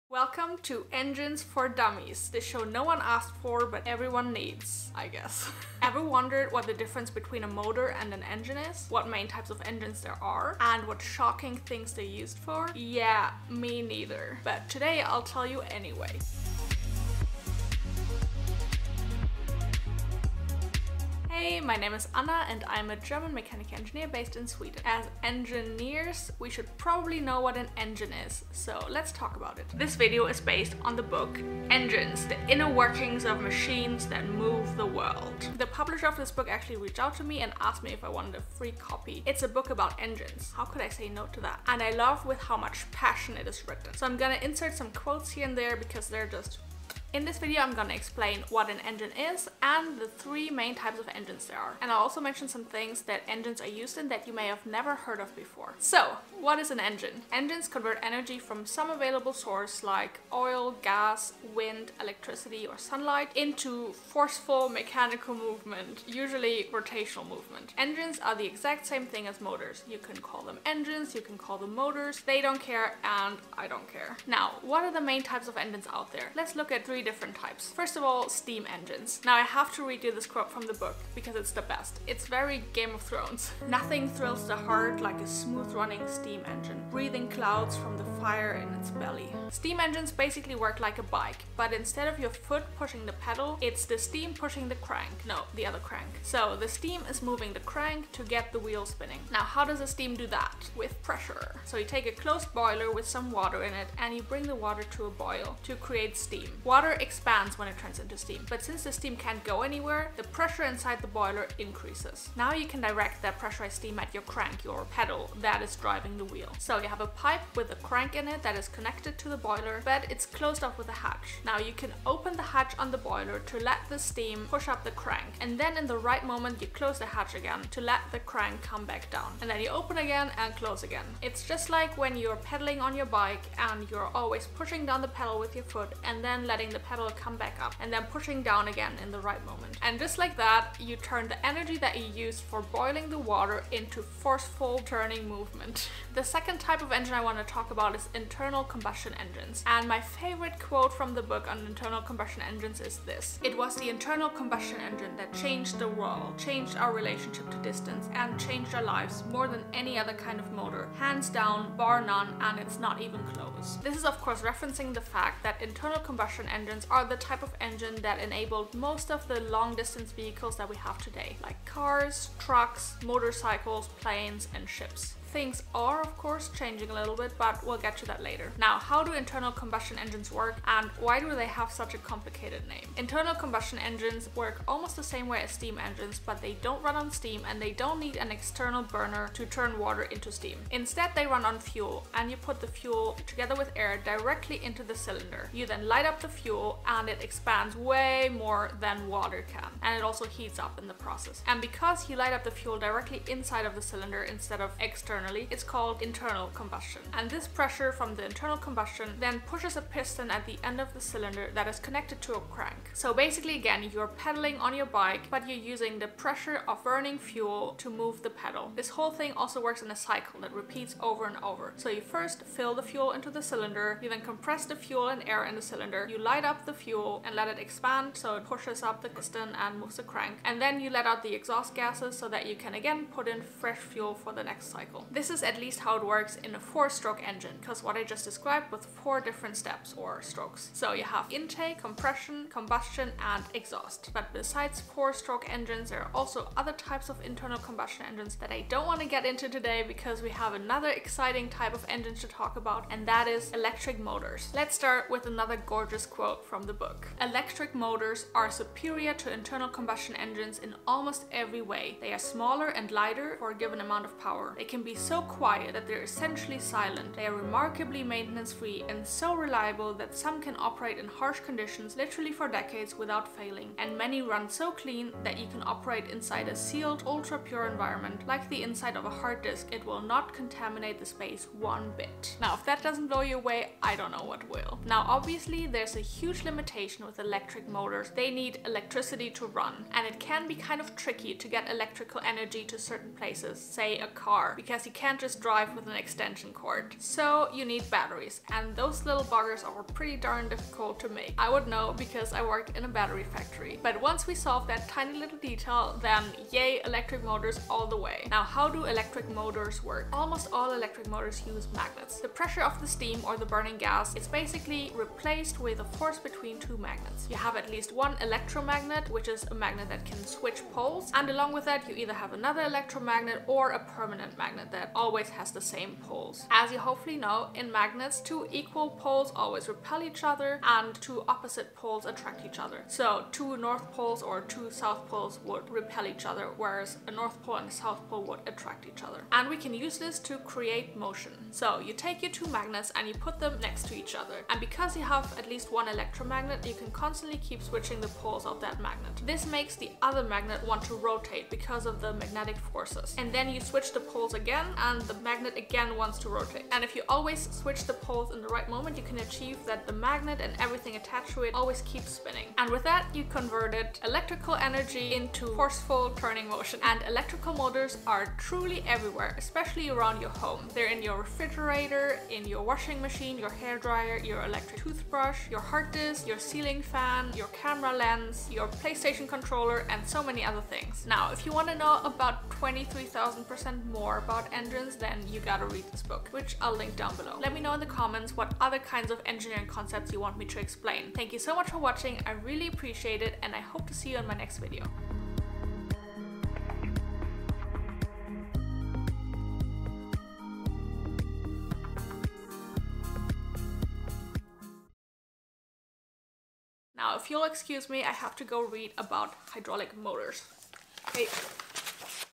How Engines Work [Beginner's Crash Course]
✨ Is it time to quit your job? Take the free quiz

TIMESTAMPS
0:00 Intro
0:56 What is an Engine?
1:14 What are the Main Types of Engines?
1:18 Steam Engines
2:26 Internal Combustion Engines
4:10 Electric Motors